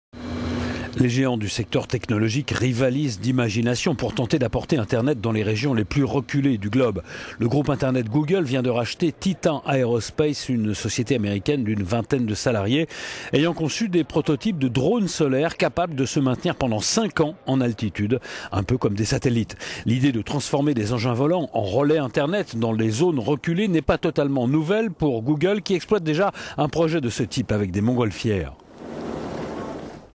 Google achète des drones solaires pour exporter internet partout sur la planète - corporate
Les géants du secteur technologique rivalisent d'imagination pour tenter d'apporter internet dans les régions les plus reculées.

Le groupe internet Google vient de racheter Titan Aerospace, une société américaine d'une vingtaine de salariés qui a conçu des prototypes de drones solaires capables de se maintenir pendant cinq ans en altitude, un peu comme des satellites.

L'idée ...